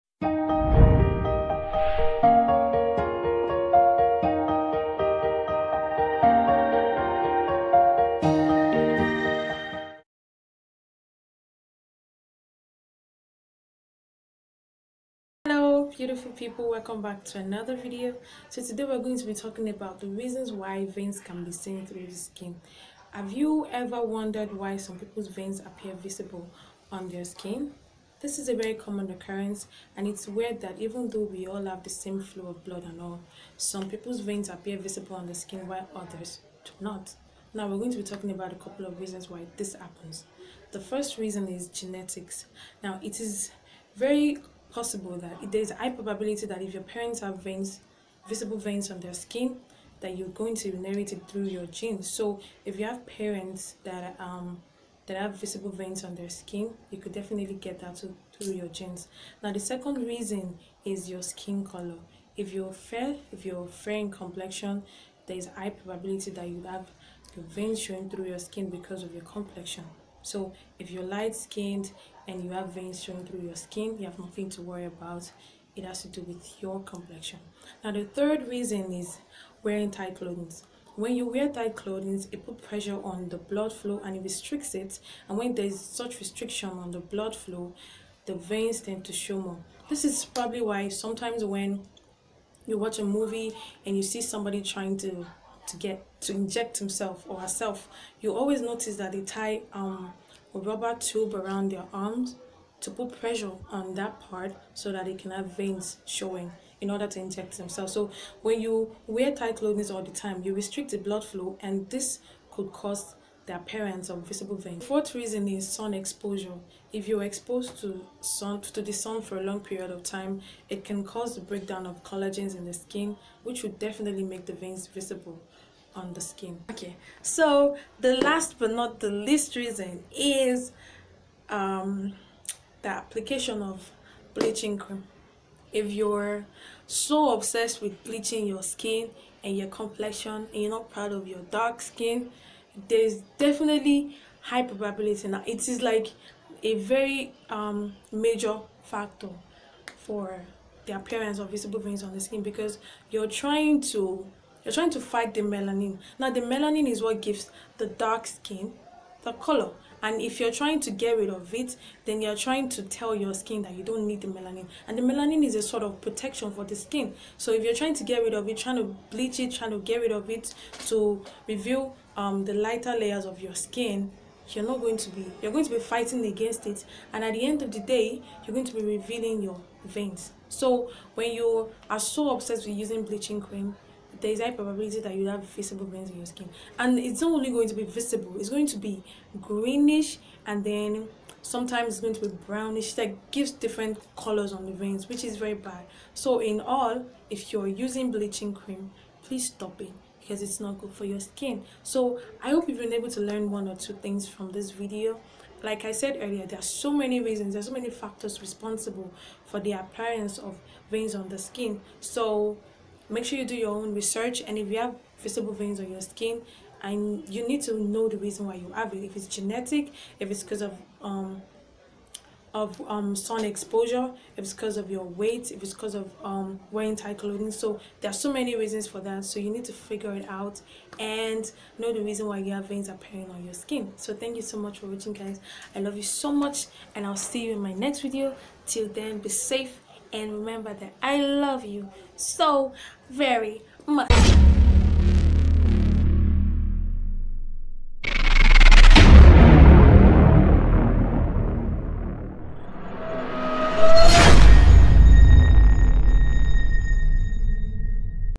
REASONS WHY YOUR VEINS ARE VISIBLE ON YOUR SKIN
There are numerous reasons for this to happen ,and if you have visible veins and you're wondering why you have it ,here are a few reasons